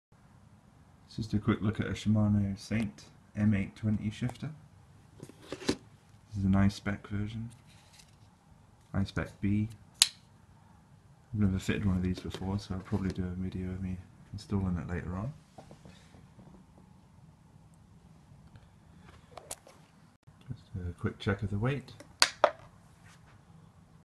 Shimano Saint M820 1x10 Speed Shifter I spec B - Unboxing
A quick unboxing of a Shimano Saint M820 1x10 Speed Shifter I spec B